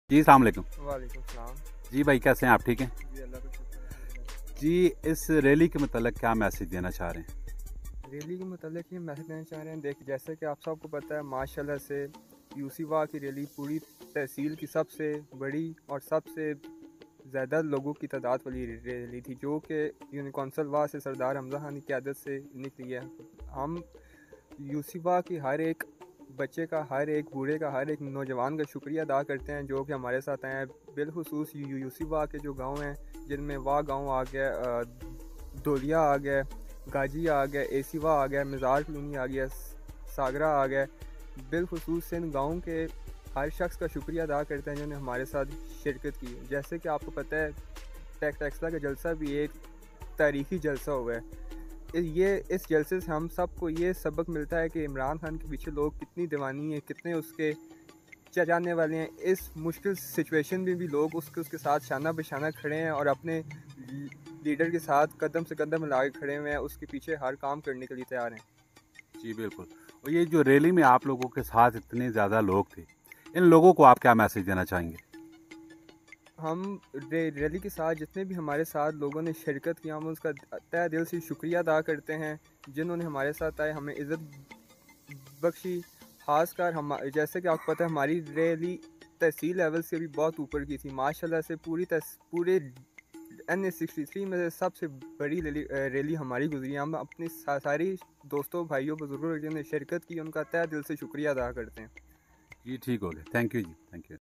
Allah Yar Khan President Union Council Wah Feedback on PTI rally |Kamran Ahmed|Daily Marketing
ہماراڈیلی مارکیٹنگ کا یوٹیوب چینل
 سبسکرائب کریں  شکریہ

ہماراڈیلی مارکیٹنگ کا  پیج لائک کریں فالو کریں شکریہ